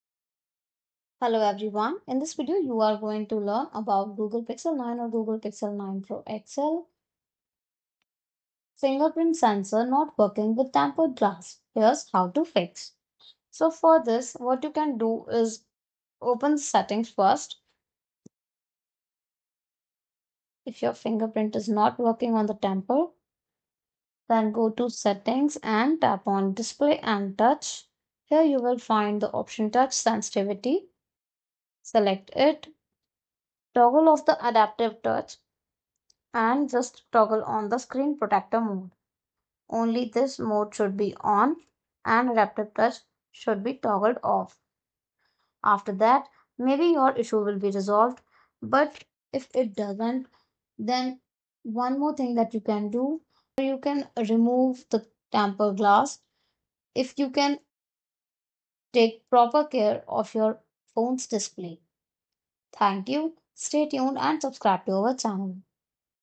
Google Pixel 9/9 Pro (XL) Fingerprint Sensor Not Working With Tempered Glass: Here's How To Fix
In this informative video, we address a common issue faced by Google Pixel 9 and 9 Pro (XL) users: the fingerprint sensor not functioning properly when a tempered glass screen protector is applied. We will guide you through effective troubleshooting steps and solutions to ensure your fingerprint sensor works seamlessly with your device. Join us as we explore practical tips to enhance your user experience and resolve this frustrating problem.